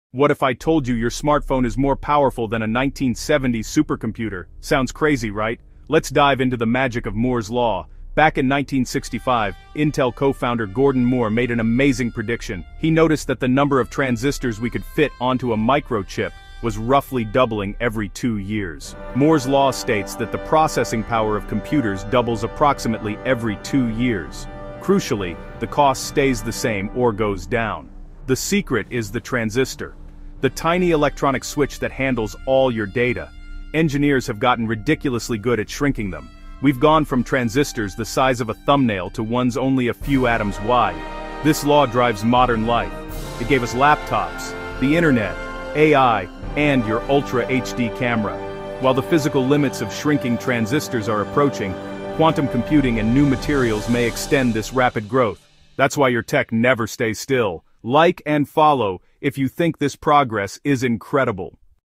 Moore's Law: Why Your Phone Gets Faster Every Year 📈
Did you know there’s a hidden rule driving the speed of technology? It’s **Moore's Law**, and it explains the exponential growth of computing power!

In 1965, Intel co-founder **Gordon Moore** predicted that the number of transistors on a microchip would double approximately every two years.

🧠 **The Impact:** This law means processing power doubles while costs remain stable or decrease. It’s driven by engineers shrinking the **transistor**—the tiny electronic switch—down to sizes just a few atoms wide!

Moore's Law is the engine behind every piece of modern tech, from AI and supercomputers to the device you’re holding right now!